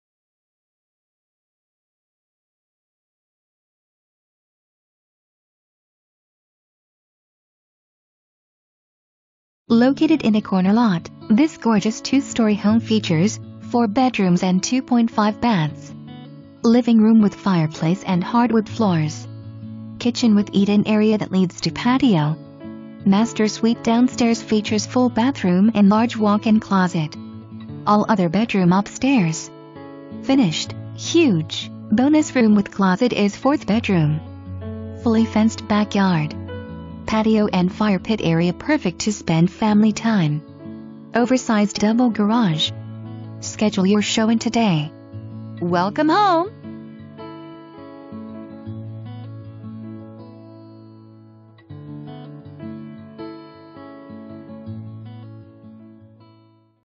1921 Yellowbrick Dr
Located in a corner lot, this gorgeous two-story home features, 4 Bedrooms & 2.5 Baths. Living Room with fireplace & hardwood floors. Kitchen with eat-in area that leads to patio. Master Suite downstairs features full bathroom & large walk-in closet. All other bedroom upstairs. Finished, HUGE, bonus room with closet is 4th Bedroom. Fully fenced backyard. Patio and fire pit area perfect to spend family time! Oversized double garage. Schedule your showing today! Welcome Home!

Presented By:
Sharon Hedgepeth, Keller Williams